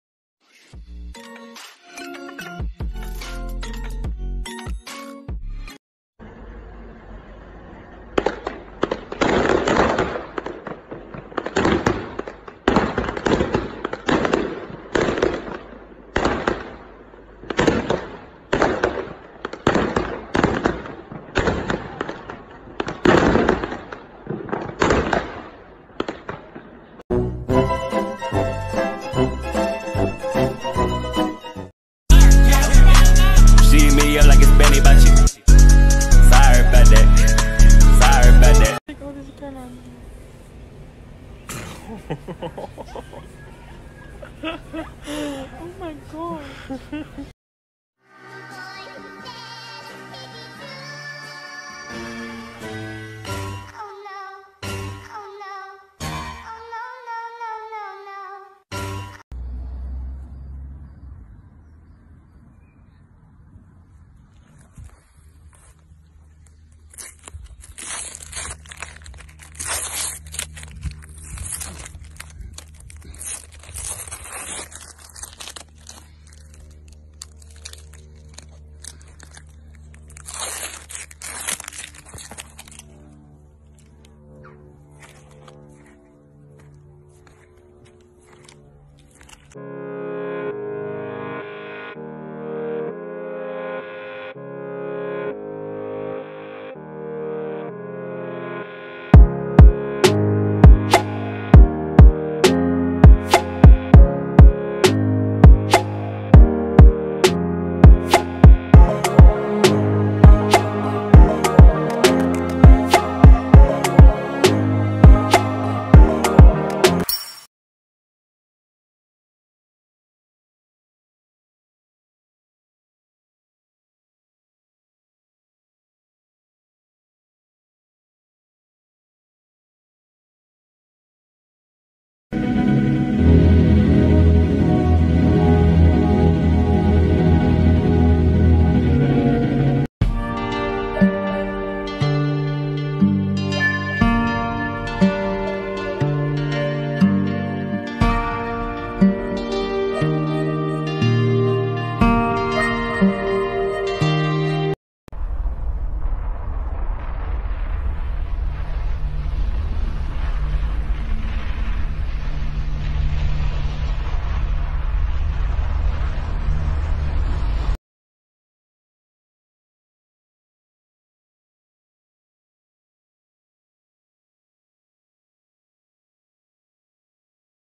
Women Uses Fireworks To Fight a Bulldozer Trying To Demolish His Home and Wins.
1.Women Fight With Bulldozer With Help Of Fireworks
2.Smoky hair
3.That was not His First time video
4.Cute Karate Boy
5.Turtle
6.Drone Video Of Building
7.Excited Dog
8.City timelaps
9.Beautiful Rain
10.Volcano